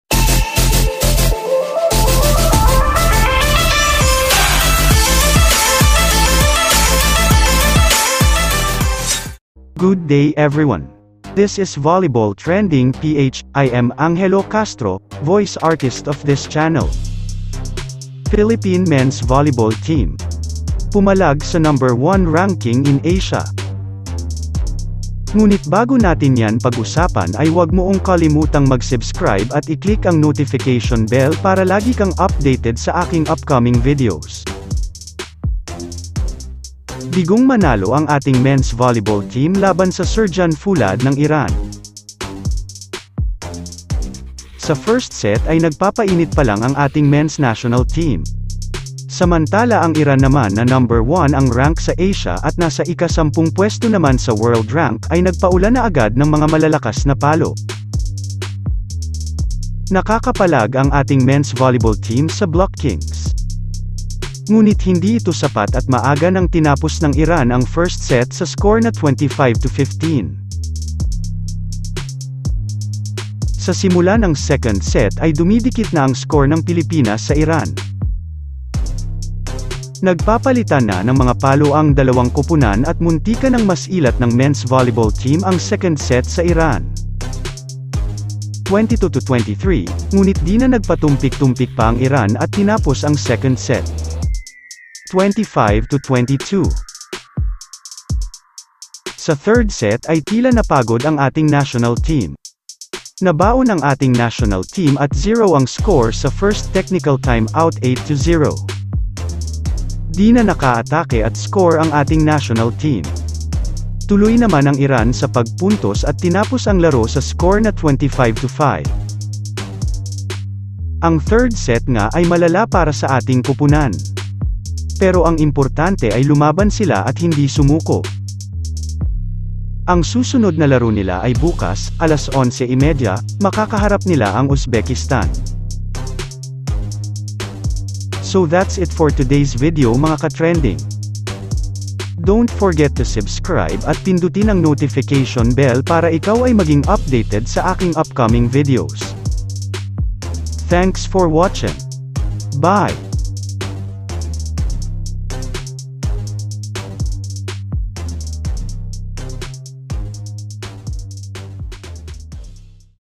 Philippine Men's Volleyball Team PUMALAG SA #1 RANK TEAM sa ASYA!
Content Editor:  Angelo D. Castro

Good Day Everyone!! This is Volleyball Trending PH… I am Angelo Castro , Voice Artist of this channel.

Philippine Men's Volleyball Team PUMALAG SA #1 RANKING IN ASIA!

Bigong manalo ang ating Men's volleyball team laban sa Sirjan Foulad ng Iran.

Sa first set ay nagpapainit palang ang ating mens national team,,, Samantala ang Iran naman na #1 ang rank sa asia at nasa ika sampung pwesto naman sa world rank ay nagpaulan na agad ng mga malalakas na palo.

Nakaka-palag ang ating mens volleyball team sa blockings.
Ngunit hindi ito sapat at maaga ng tinapos ng Iran ang first set sa score na 25-15.

Sa simula ng second set ay dumidikit na ang score ng Pilipinas sa Iran.
Nag-papalitan na ng mga palo ang dalawang kupunan at muntikan ng masilat ng  mens volleyball team ang second set sa iran.
22-23,,, ngunit di na nagpatumpik-tumpik pa ang Iran at tinapos ang second set.   25-22

Sa third set ay tila napagod ang ating national team. Na-baon ang ating national team at 0 ang score sa first technical time-out 8-0.
Di na naka-atake at score ang ating national team. Tuloy naman ang Iran sa pag puntos at tinapos ang laro sa score na 25-5.

Ang third set nga ay malala para sa ating kupunan. Pero ang importante ay lumaban sila at hindi sumuko.

Ang susunod na laro nila ay bukas,,, 11:30,,, makakaharap nila ang Uzbekistan.

Byee